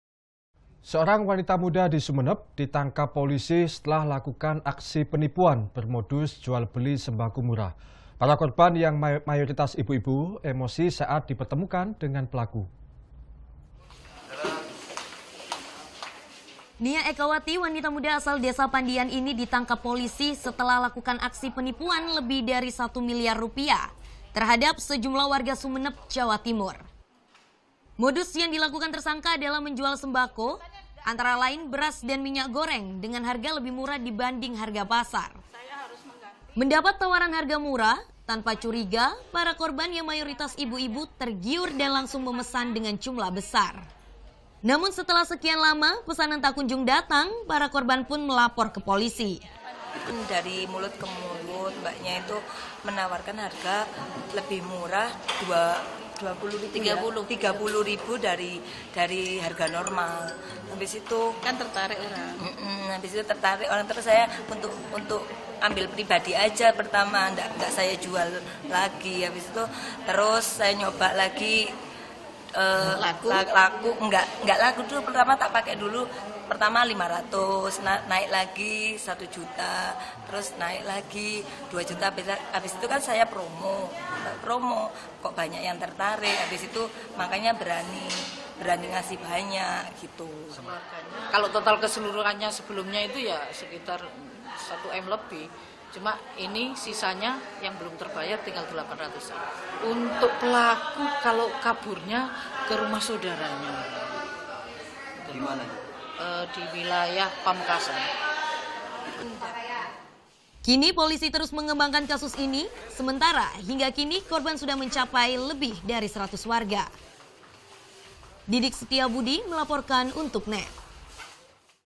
Emosinya Korban Penipuan Saat Dipertemukan Dengan Pelaku - NET. JATIM
** NET. JATIM **
Seorang wanita muda ditangkap polisi, setelah lakukan aksi penipuan bermodus jual beli sembako murah, dengan total kerugian para korban senilai 1 milyar lebih. Para korban yang mayoritas ibu - ibu, emosi saat dipertemukan dengan pelaku .